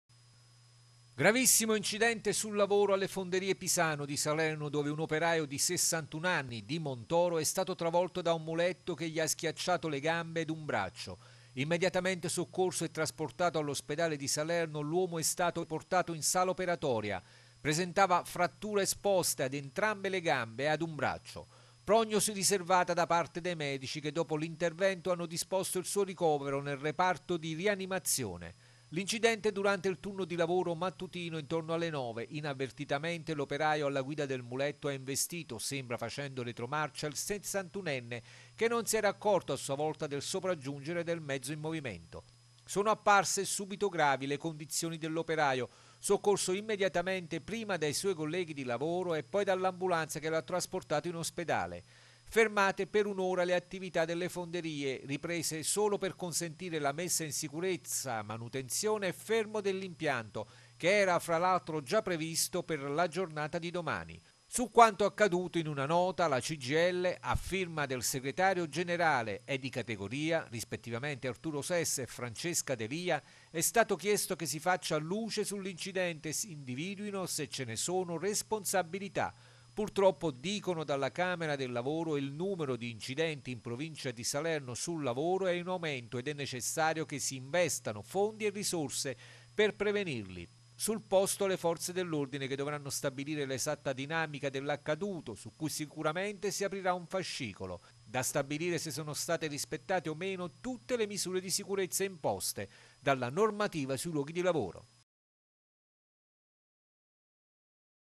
Incidente sul lavoro alle Fonderie Pisano di Salerno
Travolto da un muletto un lavoratore 61enne è ora ricoverato nel reparto di rianimazione dell'ospedale di Salerno.